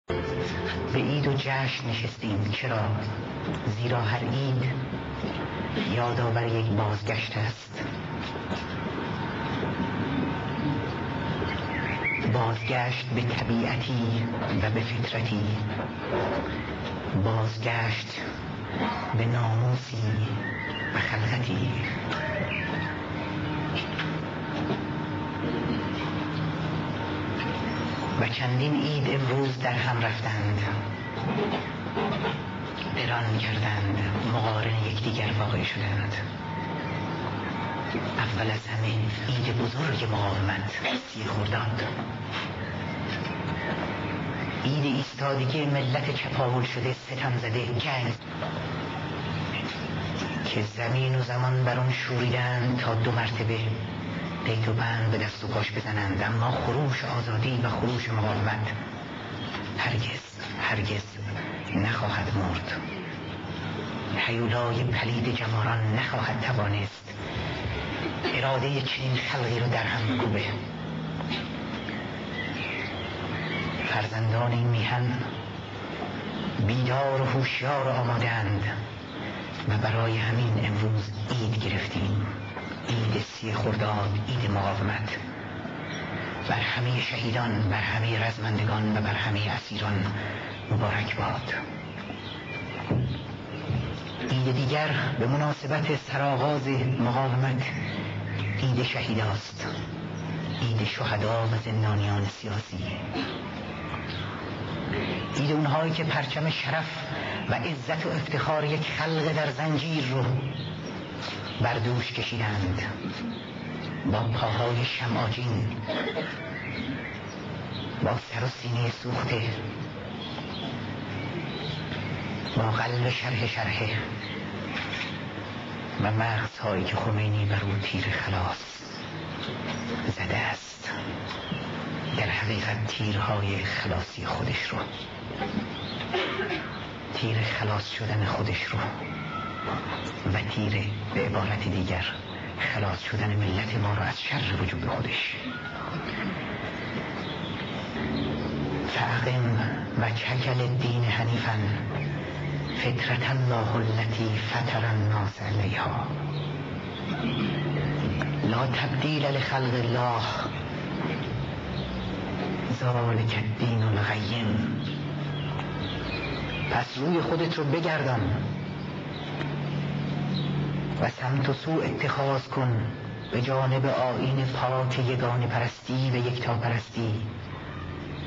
aghaye rajavi 1364
sohbat haye aghaye rajavi dar 30khordad 1364 az sima ye azadi television meli iran